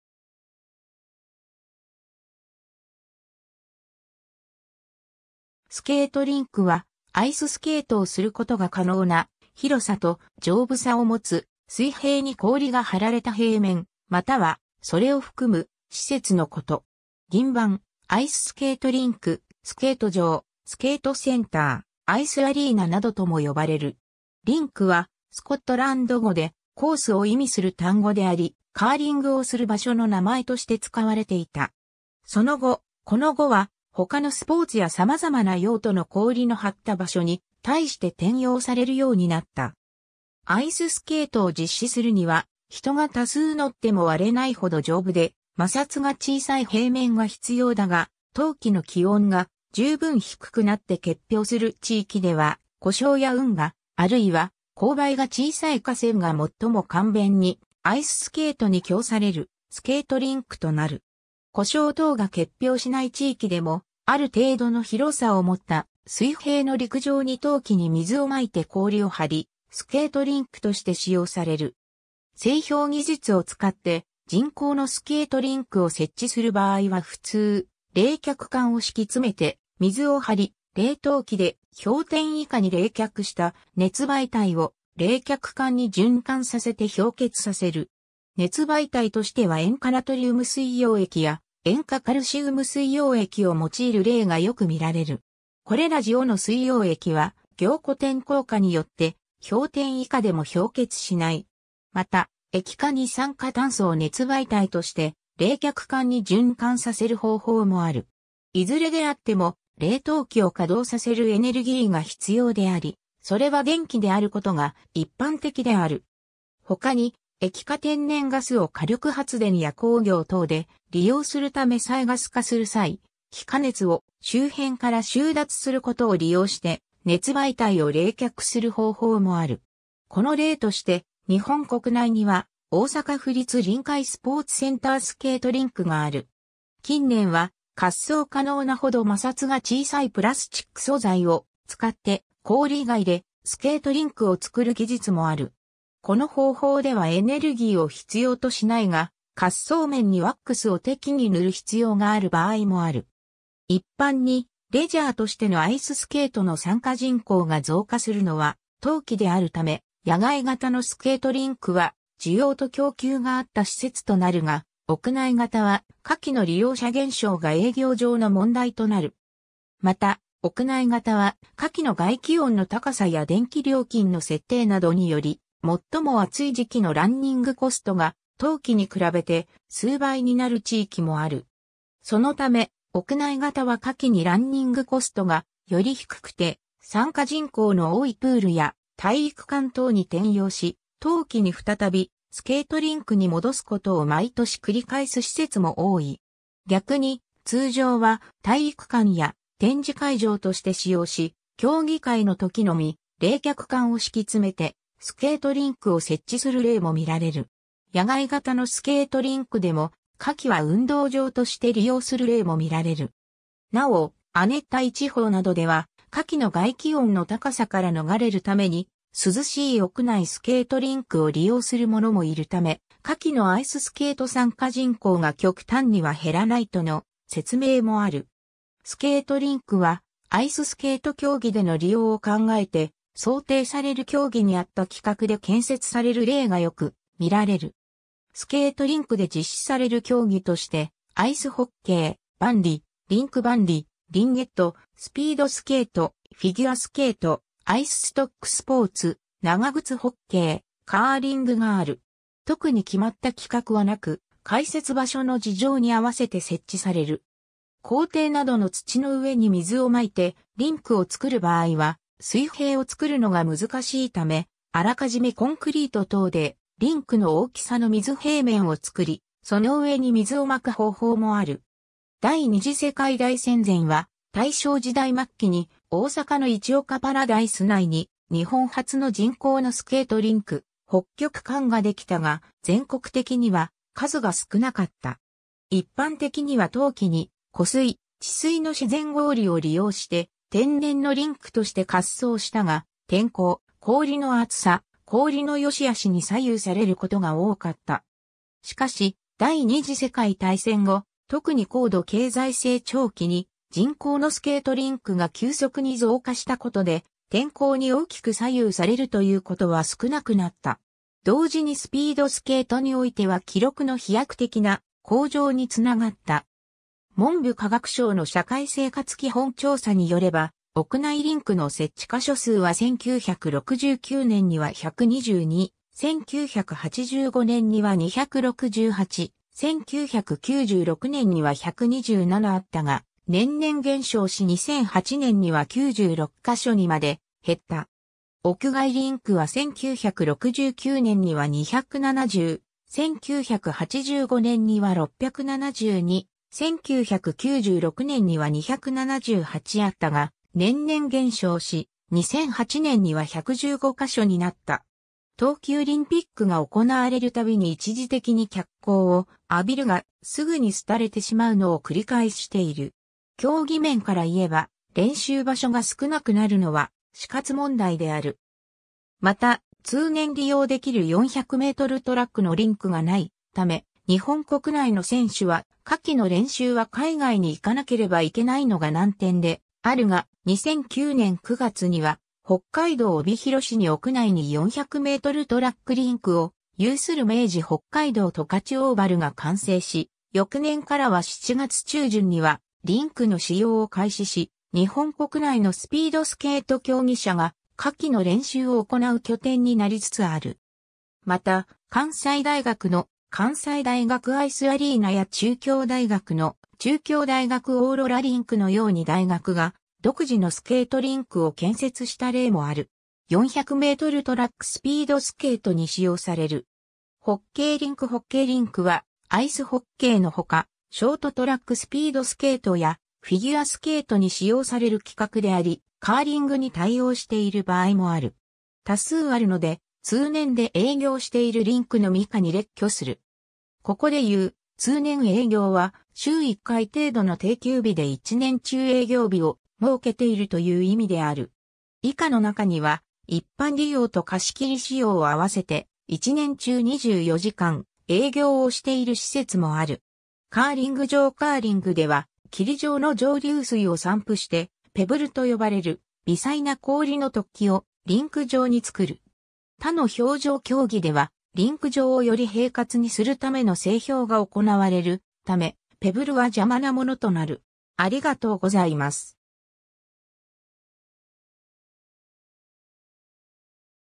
スケートリンク
スケートリンク（英: ice rink、または、(ice) skating rink）は、アイススケートをすることが可能な広さと丈夫さを持つ、水平に氷が張られた平面、または、それを含む施設のこと。
「銀盤」「アイススケートリンク」「スケート場」「スケートセンター」「アイスアリーナ」などとも呼ばれる。
 リンク（rink）は、スコットランド語で「コース」を意味する単語であり、カーリングをする場所の名前として使われていた。
その後、この語は、他のスポーツやさまざまな用途の氷の張った場所に対して転用されるようになった。
 アイススケートを実施するには、人が多数乗っても割れないほど丈夫で、摩擦が小さい平面が必要だが、冬季の気温が充分低くなって結氷する地域では、湖沼や運河、あるいは、勾配が小さい河川が最も簡便にアイススケートに供されるスケートリンクとなる。
湖沼等が結氷しない地域でも、ある程度の広さをもった水平の陸上（学校の校庭・河川敷・田畑など）に冬季に水をまいて氷を張り、スケートリンクとして使用される。
 製氷技術を使って人工のスケートリンク（パイピングリンク）を設置する場合は普通、冷却管を敷き詰めて水を張り、冷凍機で氷点以下に冷却した熱媒体を冷却管に循環させて氷結させる。
熱媒体としては塩化ナトリウム水溶液（塩水、ブライン）や塩化カルシウム水溶液を用いる例がよく見られる。
これら塩（えん）の水溶液は、凝固点降下によって氷点以下でも氷結しない。
また、液化二酸化炭素を熱媒体として冷却管に循環させる方法もある。
いずれであっても冷凍機を稼働させるエネルギーが必要であり、それは電気であることが一般的である。
他に、液化天然ガス（-162℃以下）を火力発電や工業等で利用するため再ガス化する際、気化熱を周辺から収奪することを利用して熱媒体を冷却する方法もある。
この例として日本国内には、大阪府立臨海スポーツセンタースケートリンクがある。
 近年は、滑走可能なほど摩擦が小さいプラスチック素材を使って、氷以外でスケートリンクを造る技術もある。
この方法ではエネルギーを必要としないが、滑走面にワックスを適宜塗る必要がある場合もある。
 一般にレジャーとしてのアイススケートの参加人口が増加するのは冬季であるため、野外型のスケートリンクは需要と供給が合った施設となるが、屋内型は夏季の利用者減少が営業上の問題となる。
また、屋内型は夏季の外気温の高さや電気料金の設定などにより、最も暑い時期のランニングコストが冬季に比べて数倍になる地域もある。
そのため、屋内型は夏季にランニングコストがより低くて参加人口の多いプールや体育館等に転用し、冬季に再びスケートリンクに戻すことを毎年繰り返す施設も多い。
逆に、通常は体育館や展示会場として使用し、競技会のときのみ冷却管を敷き詰めてスケートリンクを設置する例も見られる。
野外型のスケートリンクでも、夏季は運動場として利用する例も見られる。
なお、亜熱帯地方などでは、夏季の外気温の高さから逃れるために、涼しい屋内スケートリンクを利用する者もいるため、夏季のアイススケート参加人口が極端には減らないとの説明もある。
 スケートリンクはアイススケート競技での利用を考えて、想定される競技に合った規格で建設される例がよく見られる。
スケートリンクで実施される競技として、アイスホッケー、バンディ、リンクバンディ、リンゲット、スピードスケート、フィギュアスケート、アイスストックスポーツ、長靴ホッケー、カーリングがある。
 特に決まった規格はなく、開設場所の事情に合わせて設置される。
校庭などの土の上に水をまいてリンクをつくる場合は水平をつくるのが難しいため、あらかじめコンクリート等でリンクの大きさの水平面をつくり、その上に水をまく方法もある。
 第二次世界大戦前は、大正時代末期に大阪の市岡パラダイス内に日本初の人工のスケートリンク「北極館」ができたが、全国的には数が少なかった。
一般的には冬季に湖水・池水の自然氷を利用して天然のリンクとして滑走したが、天候、氷の厚さ、氷の良し悪しに左右されることが多かった。
しかし、第二次世界大戦後、特に高度経済成長期に人工のスケートリンクが急速に増加したことで、天候に大きく左右されるということは少なくなった。
同時にスピードスケートにおいては記録の飛躍的な向上につながった。
 文部科学省の社会生活基本調査によれば、屋内リンクの設置箇所数は1969年には122、1985年には268、1996年には127あったが、年々減少...